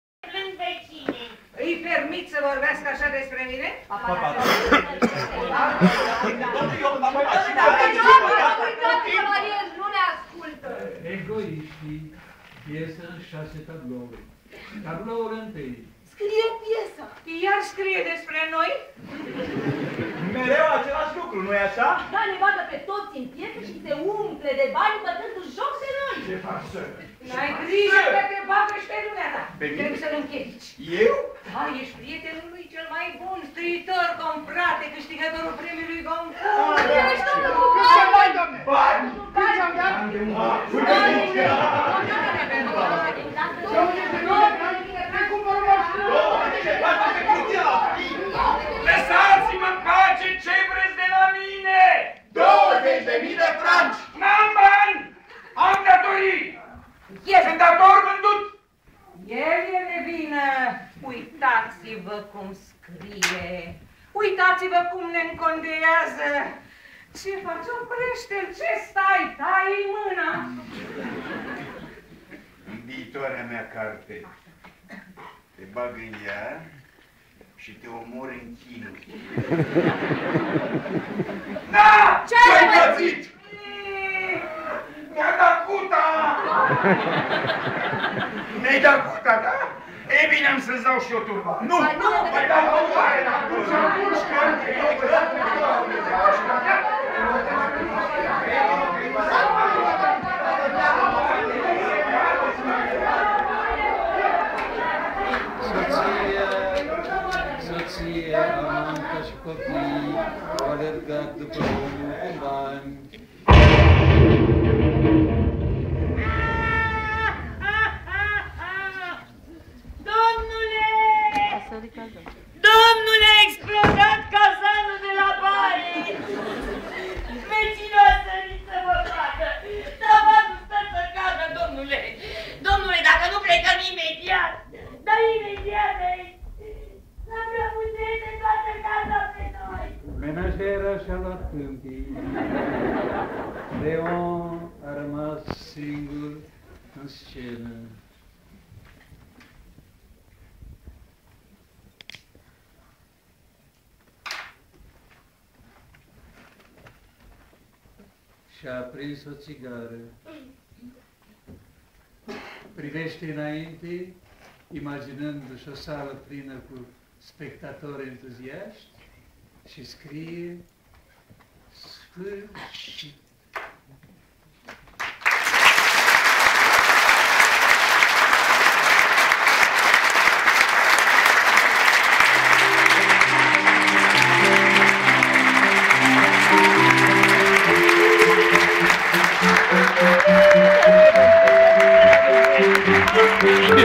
Radu Beligan-Egoistul (finalul piesei)
Finalul piesei Egoistul de Jean Anouilh, Teatrul National stagiunea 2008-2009. Cu: Radu Beligan, Carmen Stanescu\Simona Bondoc, Damian Crasmaru, Marin Moraru, Sanda Toma, Mihai Niculescu, Lamia Beligan, Medeea Marinescu/Ileana Olteanu, Cesonia Postelnicu, Silviu Biris